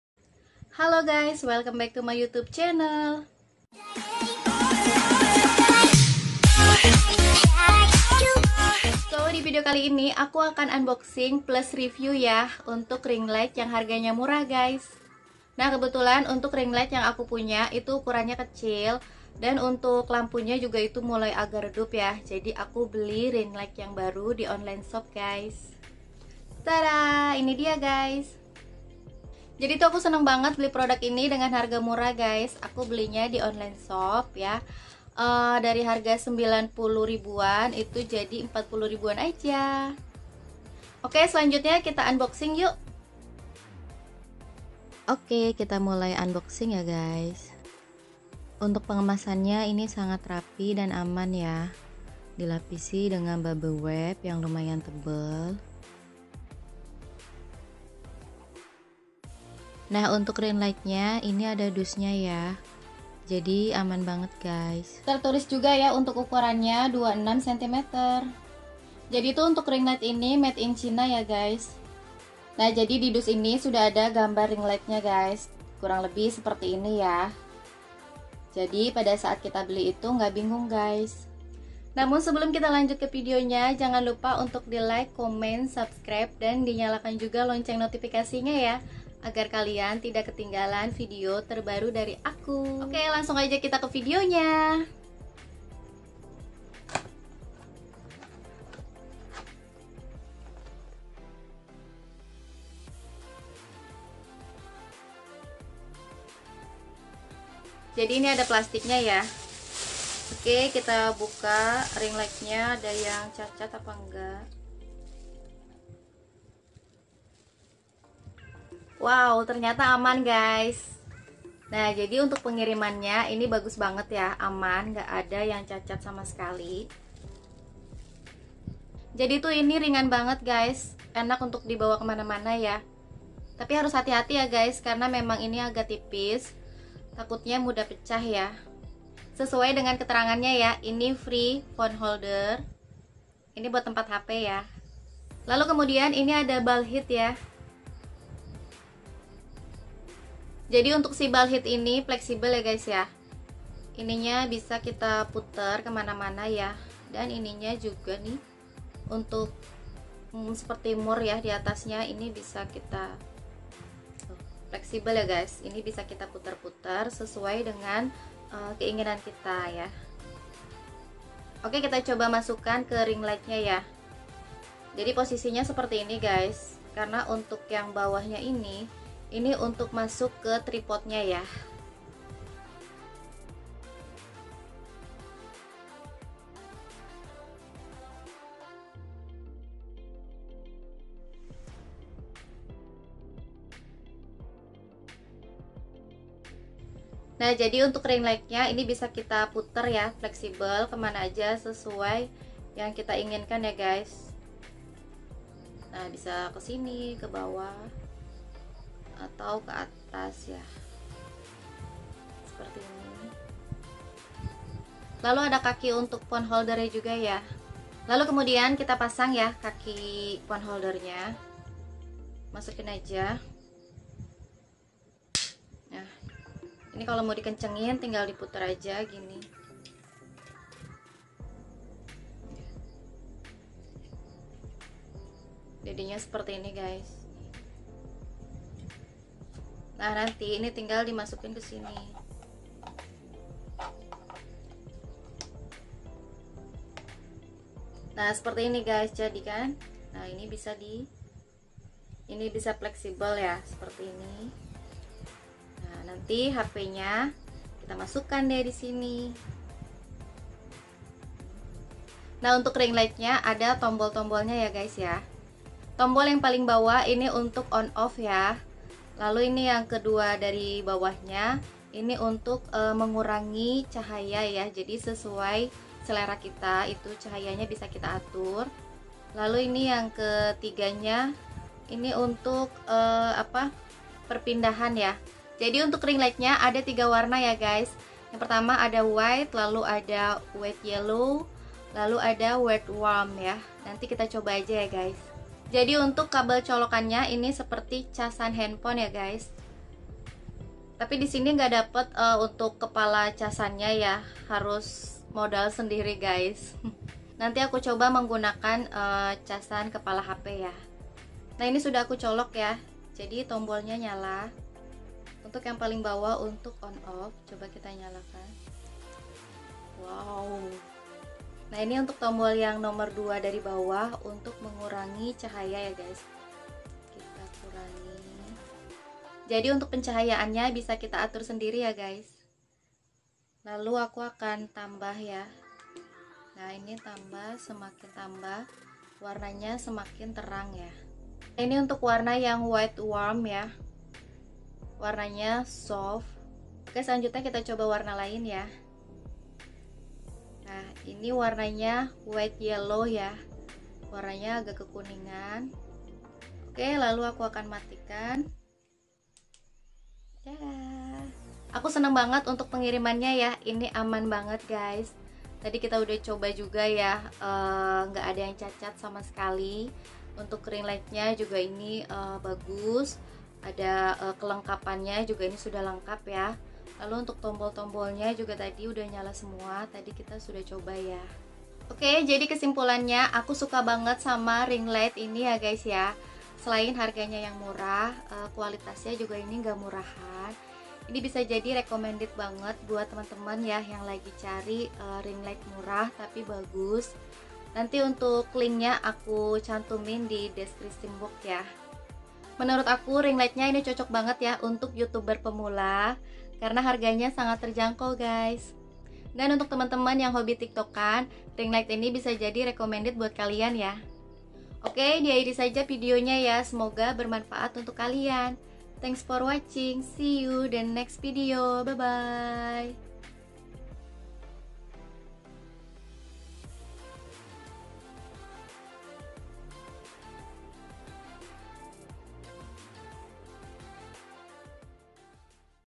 Ring Light ukuran 26cm Termurah || Unboxing + Review Ring Fill Light
halo guys... welcome back to my youtube channel, di video kali ini aq mau unboxing + review Ring Light termurah di shopee cuma 40ribuan aja udah gitu banyak free nya😱 hmm...kira2 bagus gak ya, yuks simak videonya sampai habis ya😊

Happy Watching